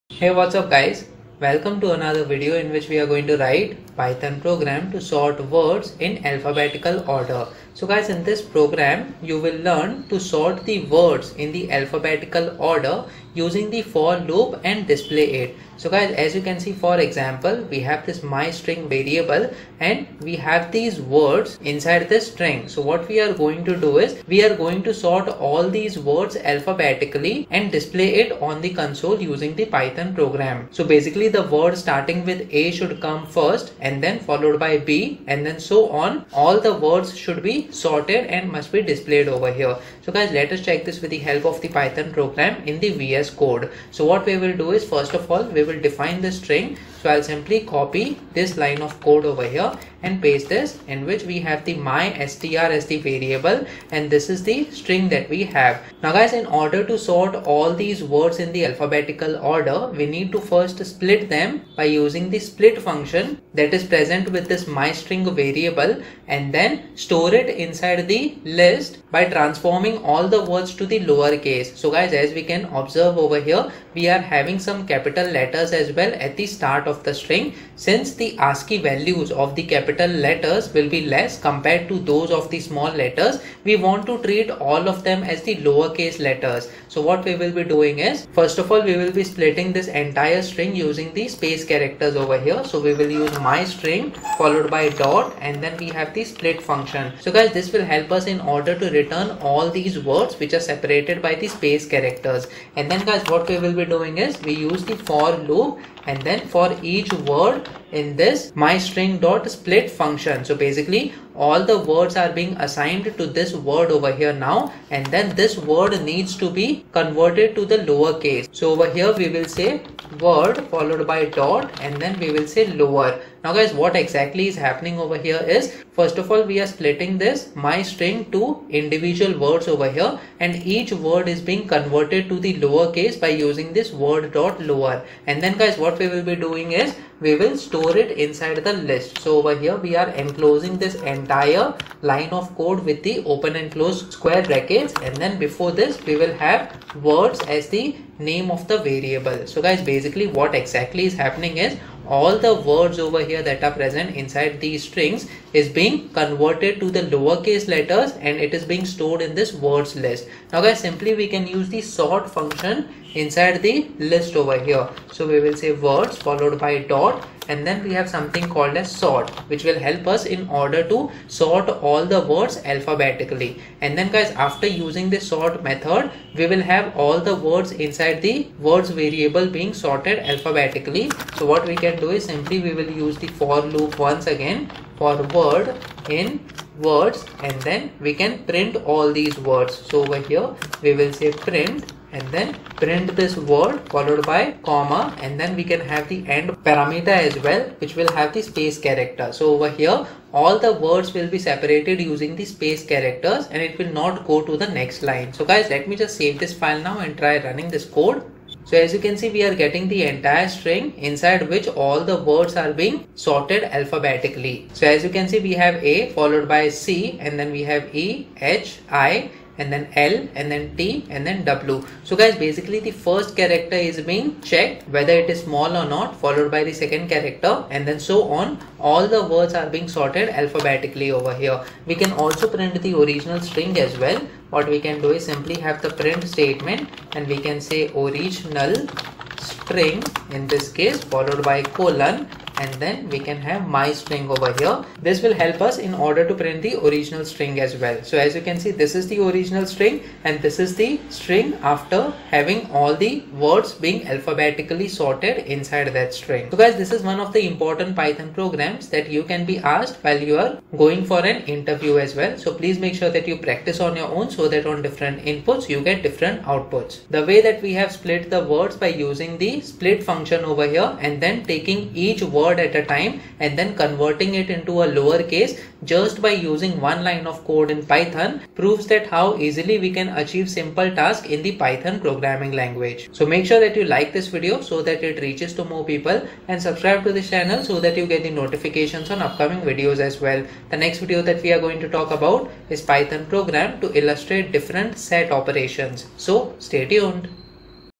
Python Program #44 - Sort Words in Alphabetic Order in Python
In this video by Programming for beginners we will see Python Program to Sort Words in Alphabetic Order in Python for beginners series.
Python programming language and program is very easy to learn for students and professionals.

In this program, you'll learn to sort the words in alphabetic order using for loop and display it.

my_str = "Hello this Is an Example With cased letters"